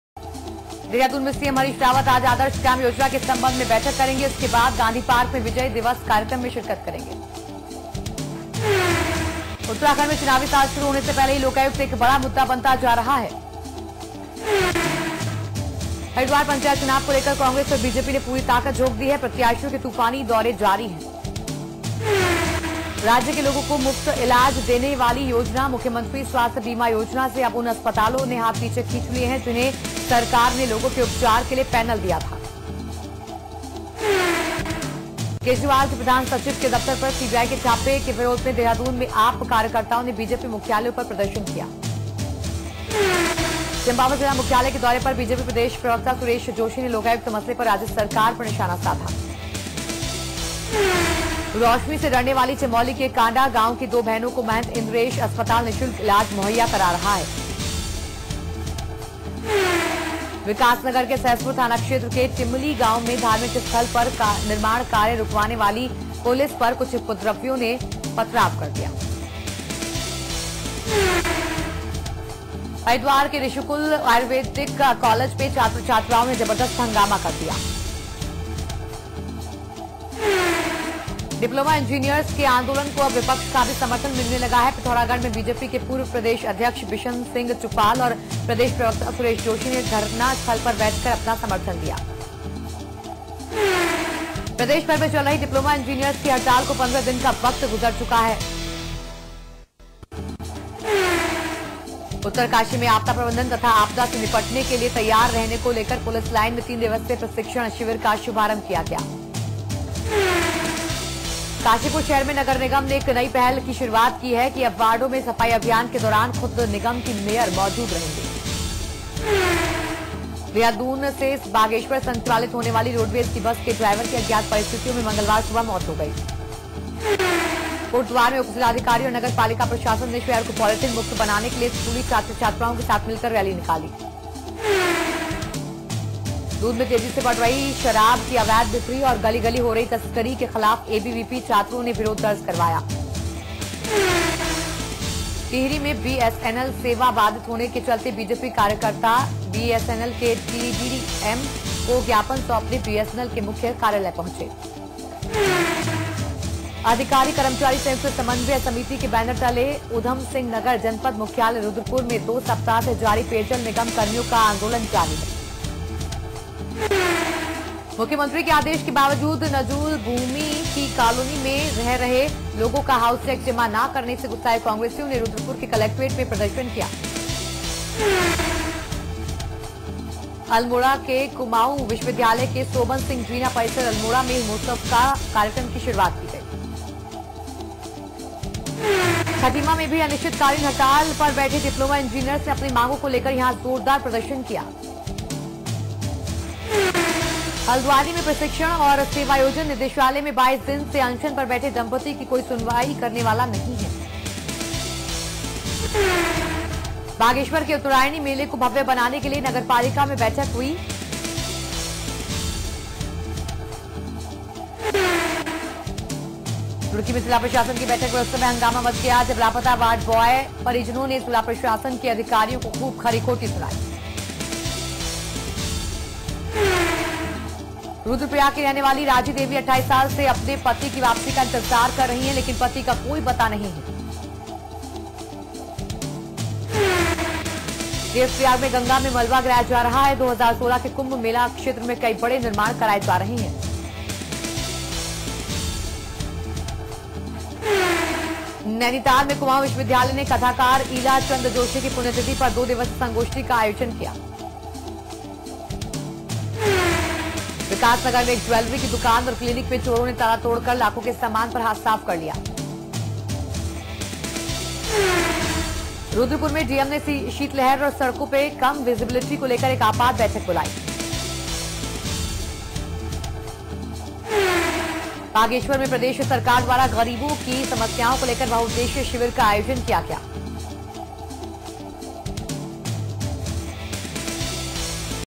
Samachar Plus: 30 News of UP's 13 districts | 16 Dec 2015
Watch the fastest news of 13 districts, Uttar Pradesh.